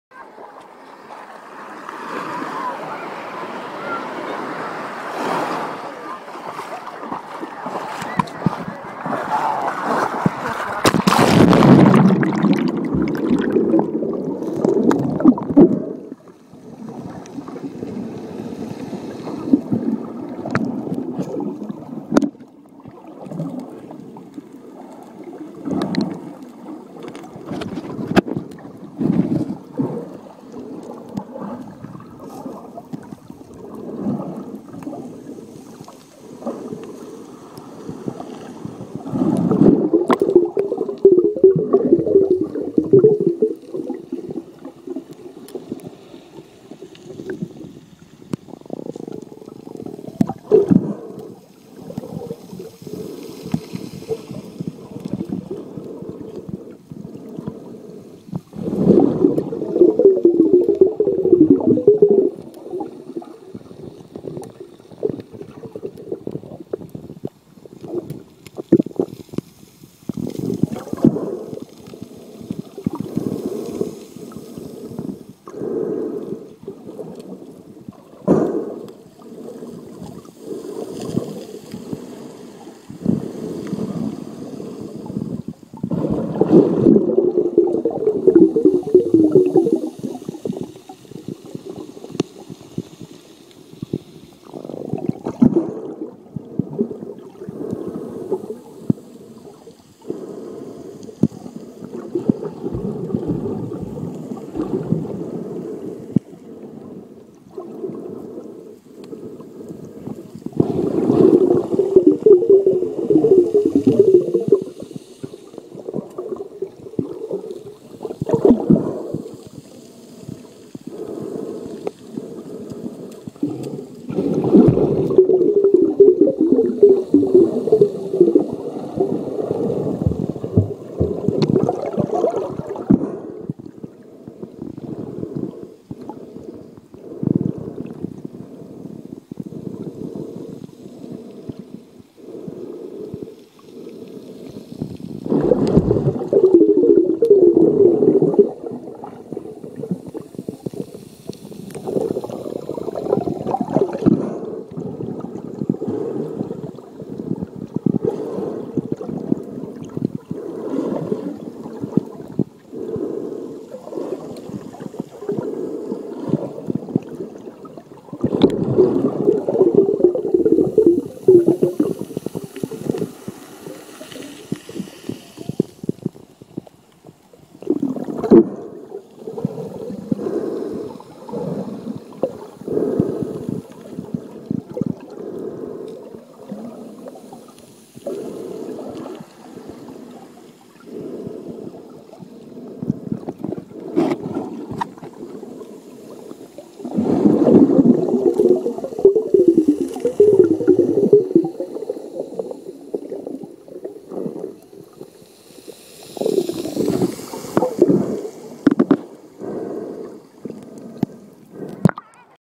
Mare di Calalunga - Montauro (CZ)
Test iphone
Primo video realizzato con questa custodia per l'iphone